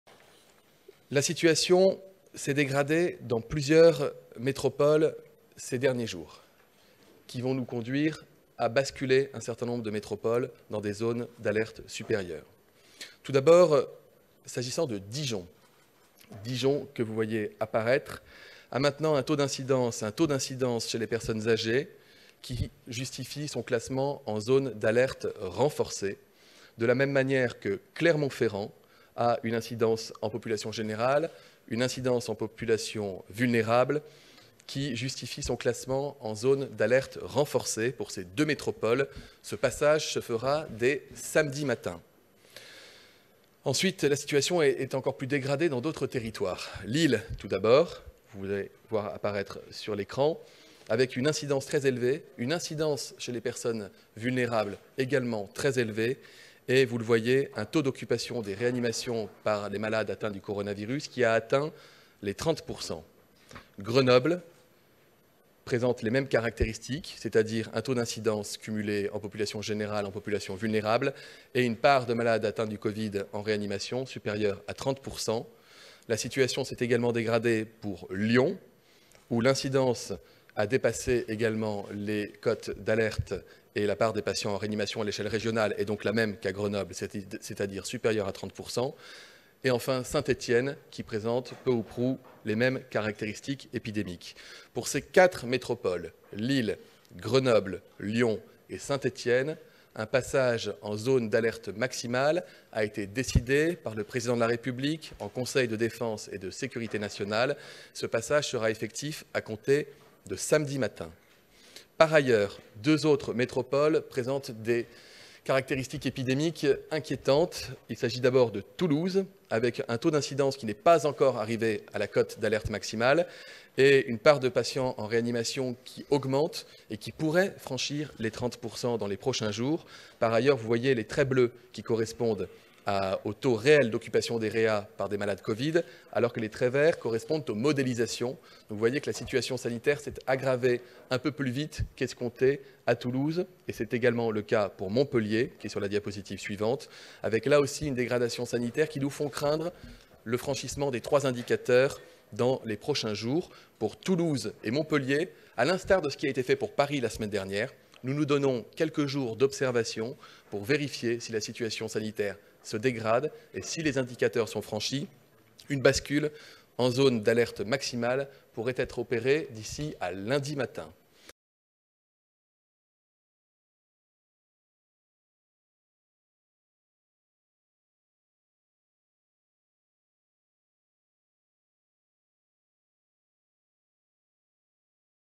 Covid-19 : Lyon, Saint-Etienne, Lille et Grenoble passent en zone d’alerte maximale
Dès samedi, ces 4 villes seront soumises aux mêmes restrictions, concernant les bars et restaurants, que Paris et Marseille.